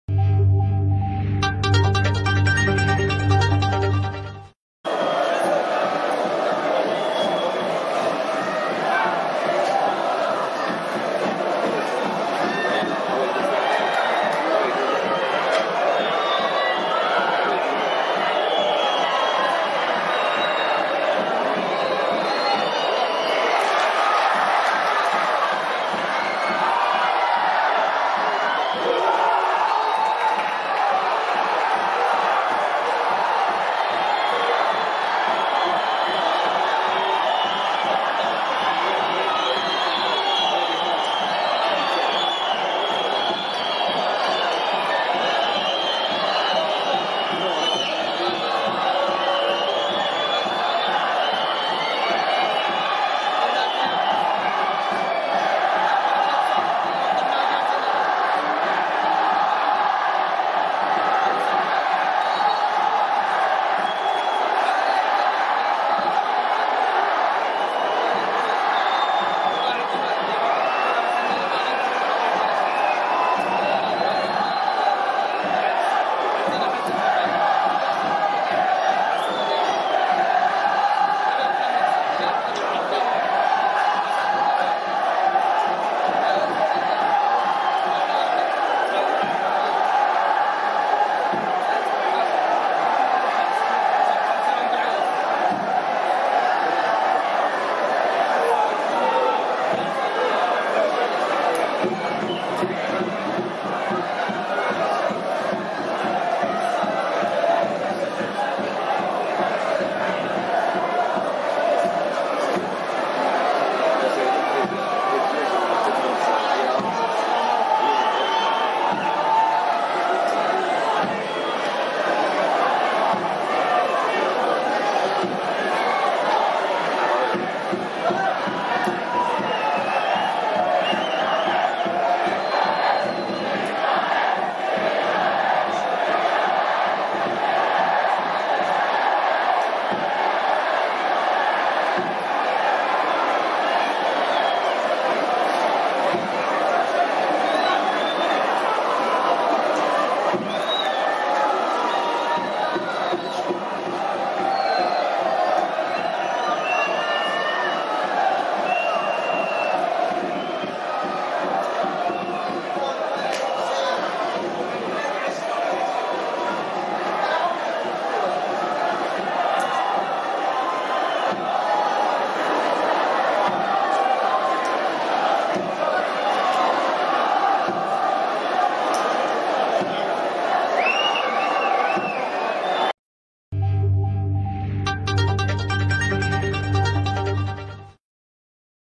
كراكاج ودادي بقلب مدرجات السعودية في مباراة الوداد ضد الهلال السعودي في كأس العرب
جمهور لا يعرف المستحيل..كراكاج ودادي بقلب مدرجات السعودية في مباراة الوداد ضد الهلال السعودي في كأس العرب.